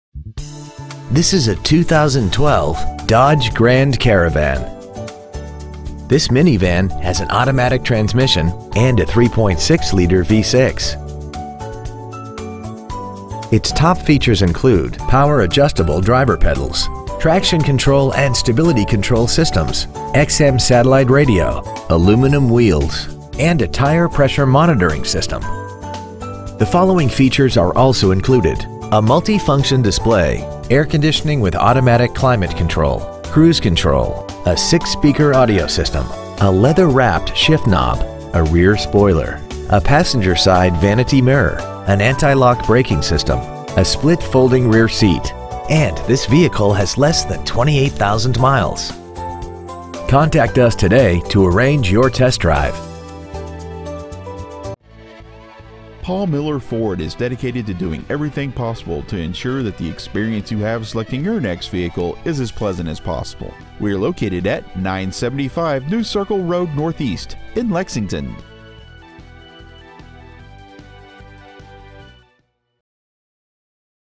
2012 DODGE GRAND CARAVAN Lexington KY
Year : 2012
 Make : DODGE
 Model : GRAND CARAVAN
 Engine : 3.6L V6 24V MPFI DOHC FLEXIBLE FUEL
 Trans . : AUTOMATIC
 Exterior : BLACK
 Miles : 27,638

 Stock : CEB04288B

 975 New Circle Road Ne